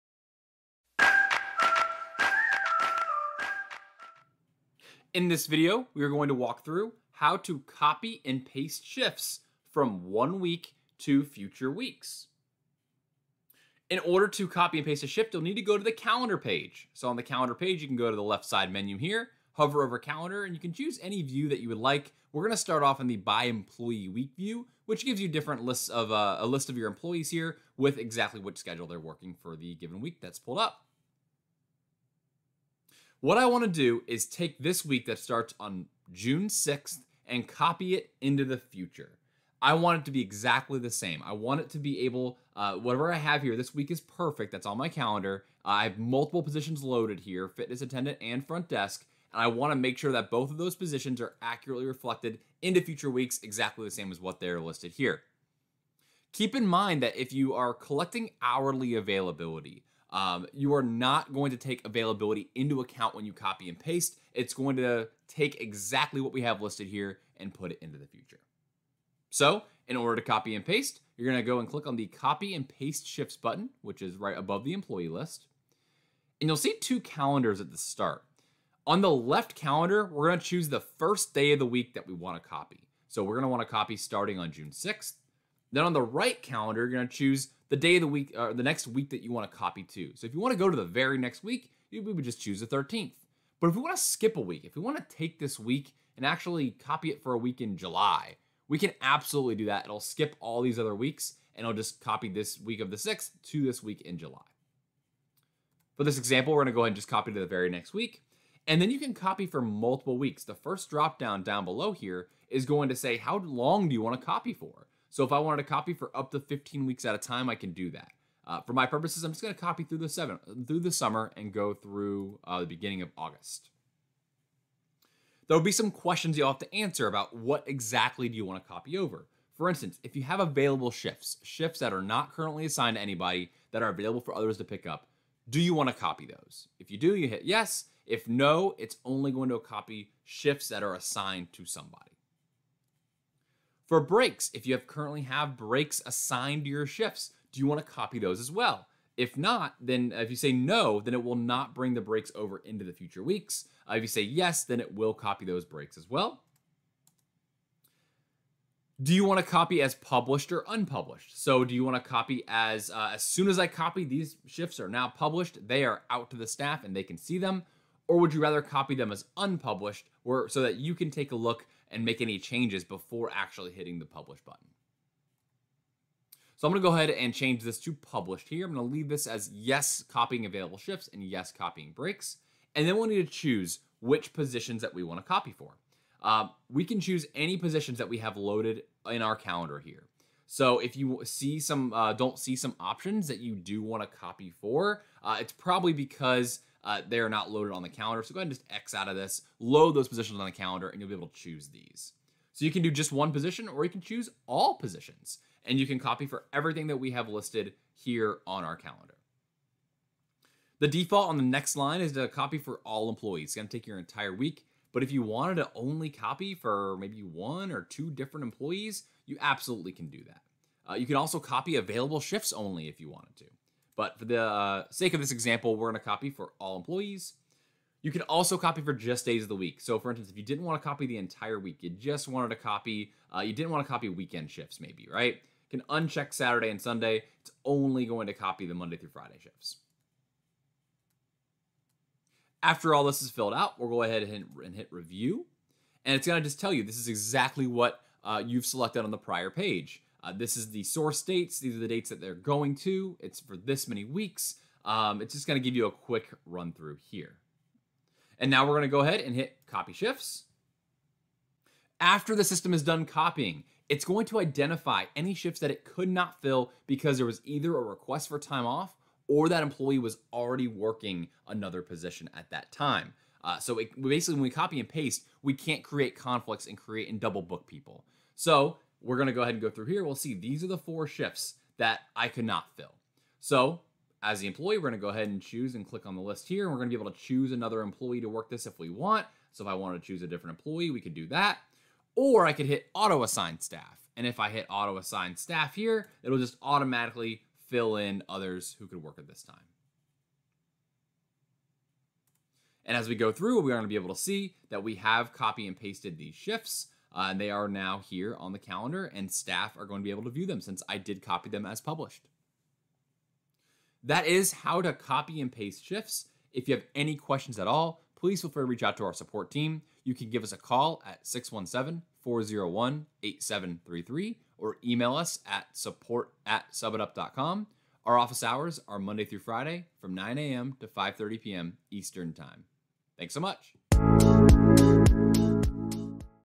How to copy and paste schedules and shifts for multiple weeks
Do you like to keep your schedule the same each week? If so, the copy and paste shift tool can save you a ton of time when creating your schedule.